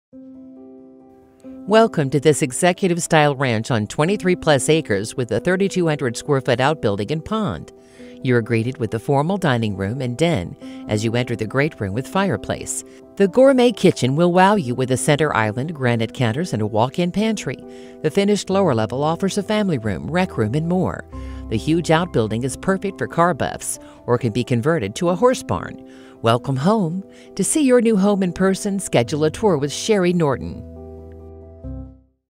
12800 State Highway M Wright City, MO | CBGundaker.com
12800 State Highway M, Wright City, MO

Welcome to this fabulous executive style ranch on 23 plus acres - plus 3,200 square ft outbuilding!

This custom home has 2,657 square ft main level and 1,100 square ft finished in walk-out lower level!

You are greeted to the elegant foyer with wood flooring leading to French doors into den and o